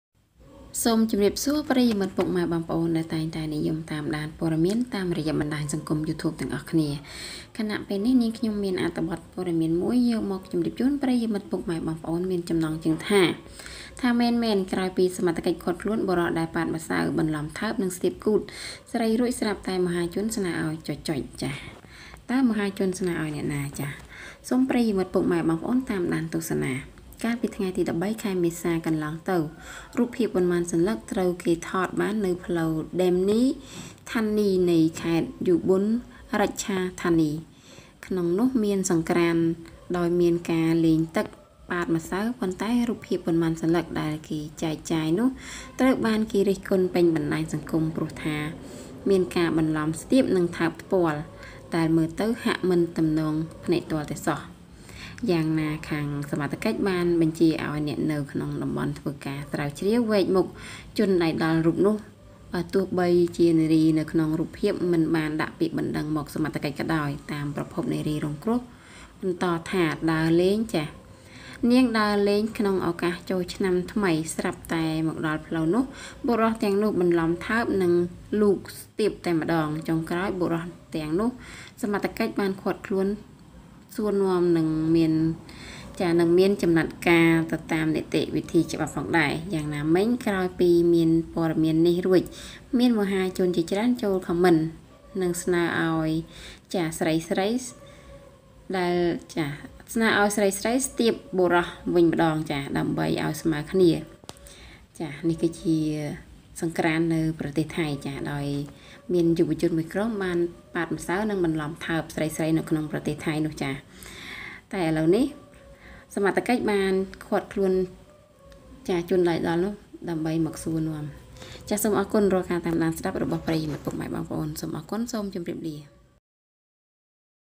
ថាមែនៗ! ក្រោយពីសម្ថកិច្ចឃាត់ខ្លួន បុរសដែលប៉ាតម្ស៉ៅបន្លំថើបនិងស្ទាបគូថស្រីរួច ស្រាប់តែមហាជនស្នើរអោយ……
That's right! After the arrest, the man who forgot to cheat, kiss and touch the female rectal, suddenly people ask to .!

បើស្រលាញ់ពេញចិត្តឆាណែលយើងខ្ញំុសូមកុំភ្លេចជួយចុចសាប់ស្រ្កែបឡាចនិងស៊ែរដើម្បីទទួលបានពត៌មានថ្មីៗទៀត។
សូមអរគុណ!